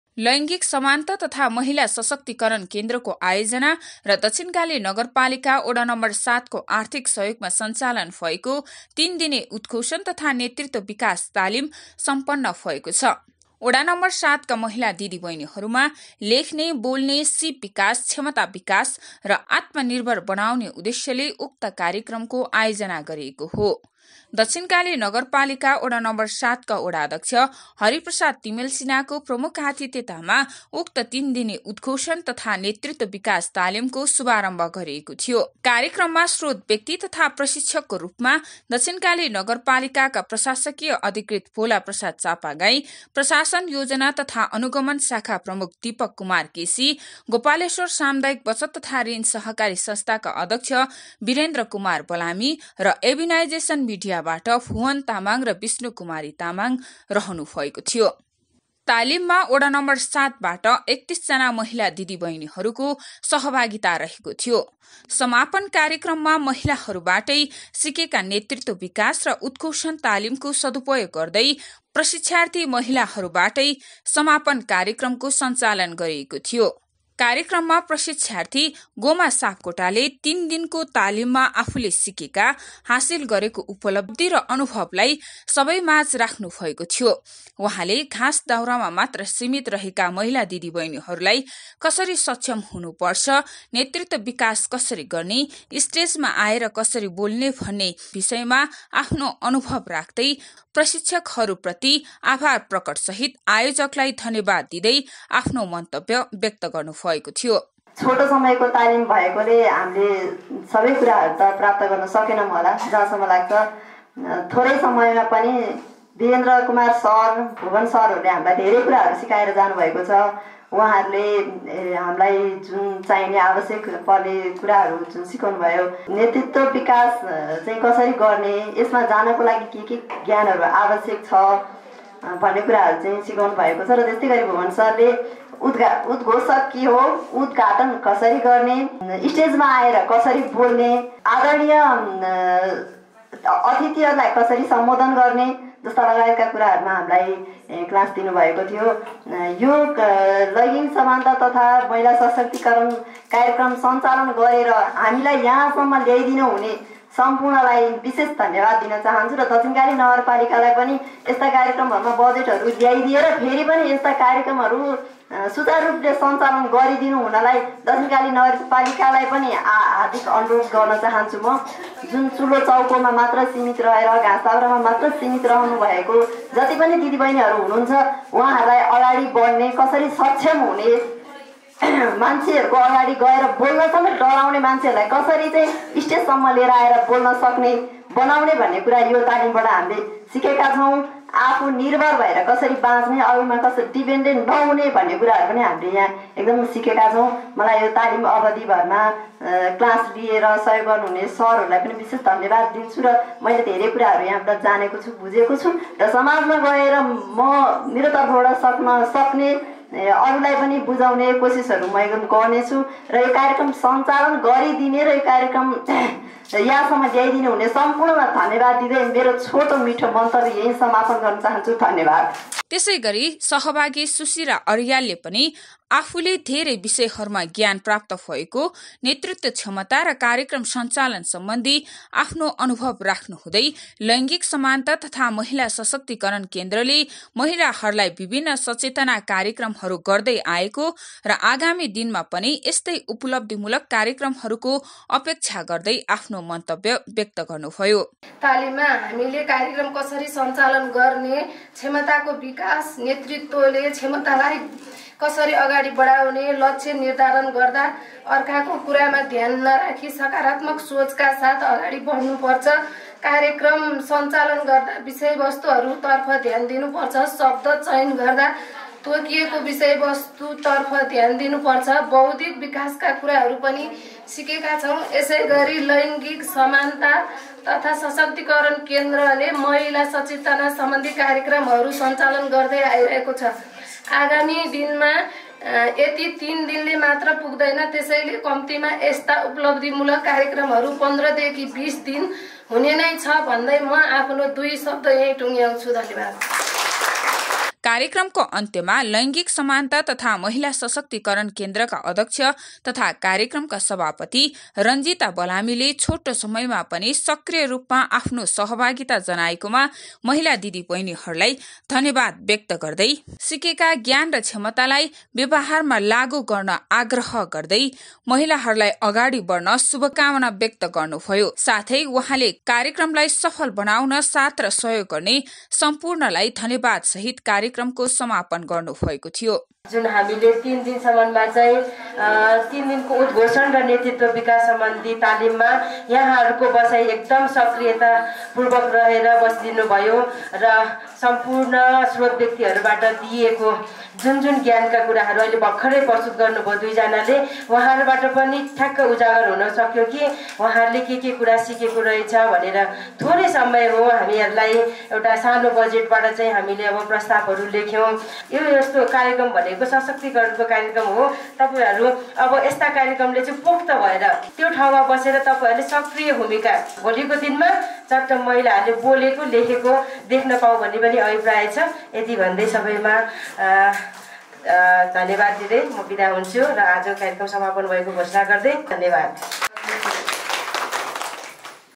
महिलाहरुले लिए ३ दिने उद्घोषण र नेतृत्व विकास तालिम, आफैले सञ्चालन गरे समापन कार्यक्रम
दक्षिणकाली नगरपालीका वडा ७ का महिलाहरुले लिए ३ दिने उद्घोषण र नेतृत्व विकास तालिम, आफैले सञ्चालन गरे समापन कार्यक्रम